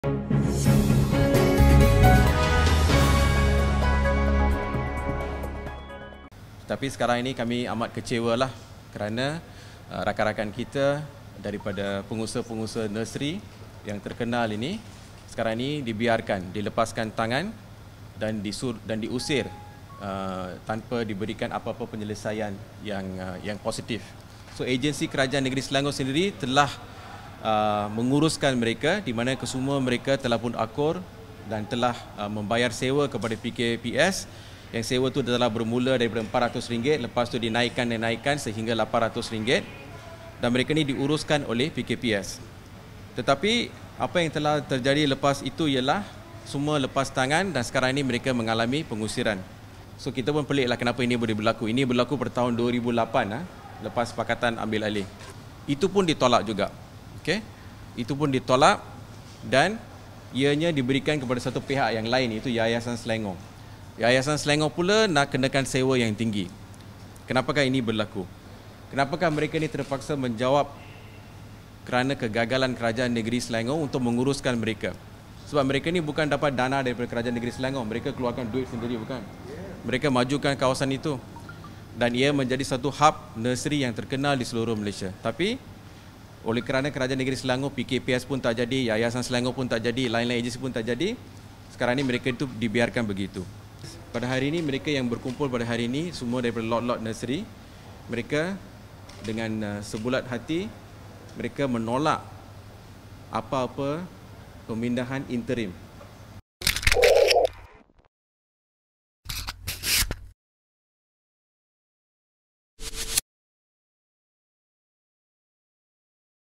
TERKINI : Kegagalan Kerajaan Negeri Selangor, Pengusaha Nurseri Jadi Mangsa - Sivaranjan
Mengecewakan sekali apabila agensi-agensi kerajaan yang pernah cuba mengendalikan pengusaha-pengusaha nurseri sepanjang jalan Sg Buloh-Shah Alam selama hampir 30 tahun kini berdepan pula dengan tindakan pengusiran.

Golongan ini dianggap penceroboh walhal mereka pernah bernaung di bawah Perbadanan Kemajuan Pertanian Selangor (PKPS) malah pernah diarahkan membayar sewa antara RM400 pada awal kemudian dinaikkan sehingga mencecah RM800 oleh agensi itu

Para pengusaha ini cuba memohon lesen pendudukan sementara (TOL) untuk sekian lama melalui PKPS namun tidak berjaya.